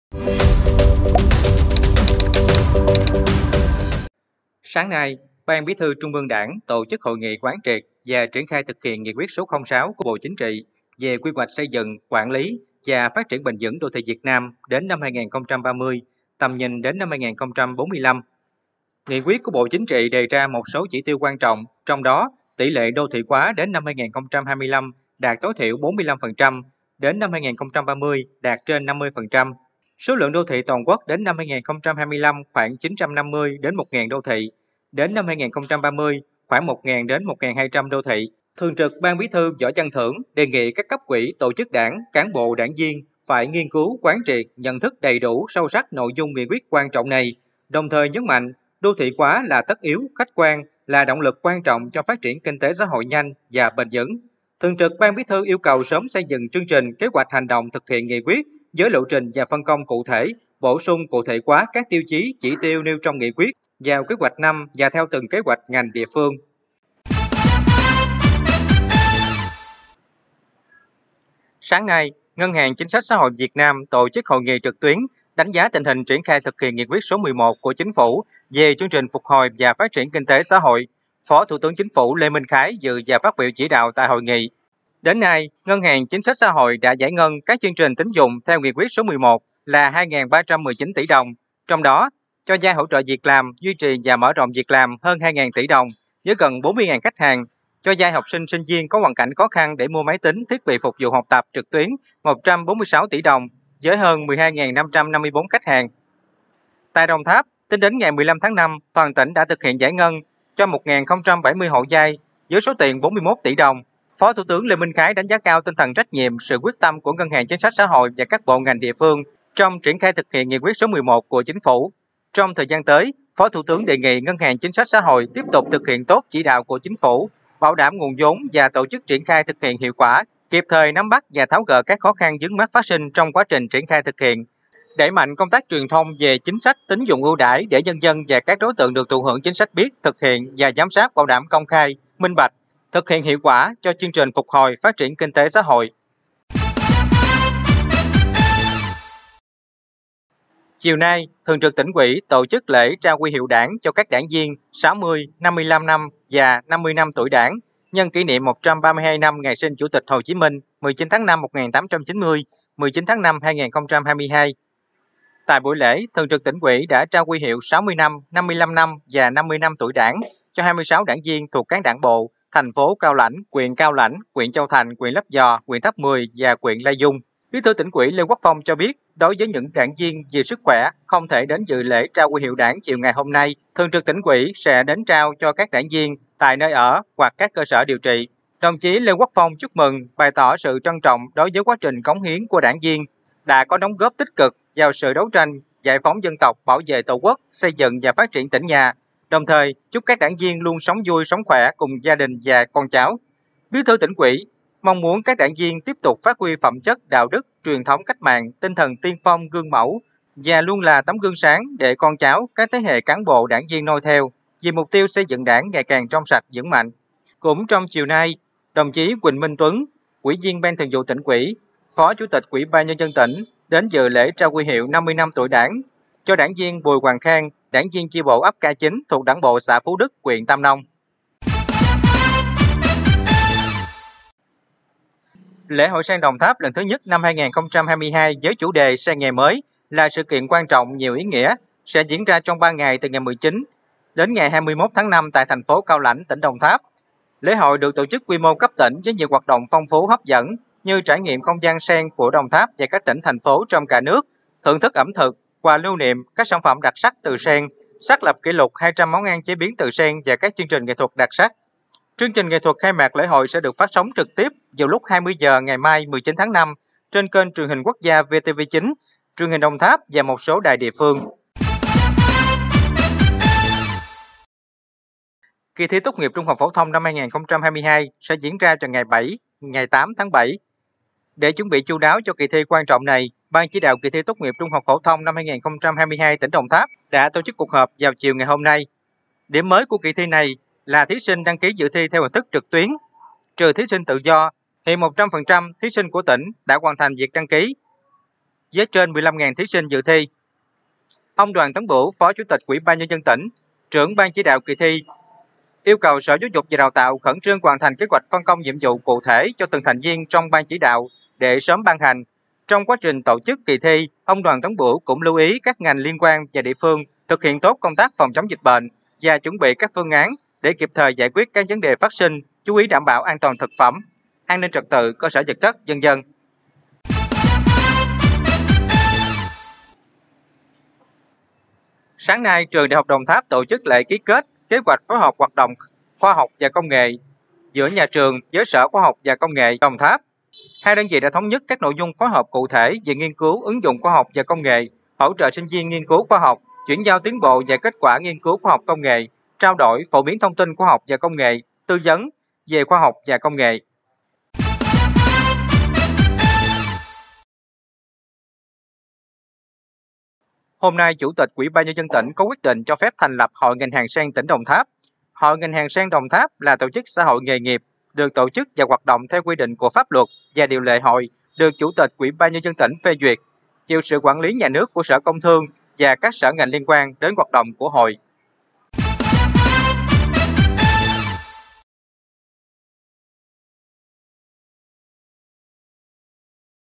Điểm tin online ngày 18/5/2022
Điểm tin ngày 18/5 có các thông tin nổi bật sau:  Đến năm 2030, cả nước có khoảng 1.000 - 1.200 đô thị;  Trao Huy hiệu Đảng cho đảng viên nhân kỷ niệm sinh nhật Bác; Giải ngân hơn 2.300 tỷ đồng theo Nghị quyết 11 của Chính phủ;  Đồng Tháp: Trên 15.000 thí sinh dự thi tốt nghiệp THPT năm 2022;  Ngày mai (19/5), khai mạc Lễ hội Sen Đồng Tháp lần thứ I - năm 2022; Trường Đại học Đồng Tháp ký kết phối hợp với Sở Khoa học và Công nghệ Đồng Tháp; Thành lập Hội Ngành hàng Sen Đồng Tháp.